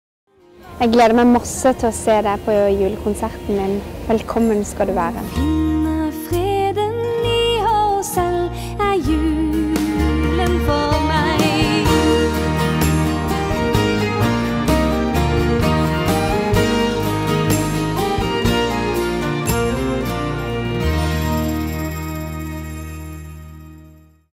20. desember
Helene Bøksle – Julekonsert den 20. desember i Vang kirke, Hamar